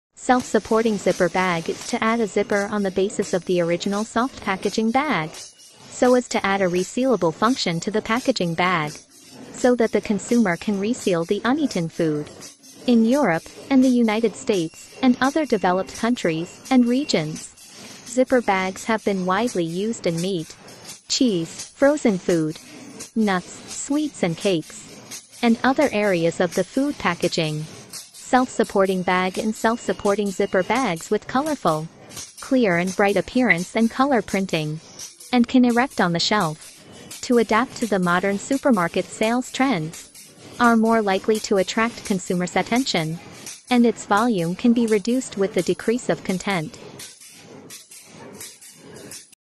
OEM plastic stand up pouch zipper pouch production video
Doypack with zipper for Snack,nut,dry food
100ml,250ml,500ml,1L,5L,etc
Every size,design,material are available
Material: Laminated Material,PET/VMPET/PE
Color:Vivid custom printing,up to 13 colors
Handle hole&open notches and other requirements are available
Feature: Good barrier property against air, moisture&puncture.
Processing Type: Multiple Extrusion
Transparency: Opaque
Brand Name: ouji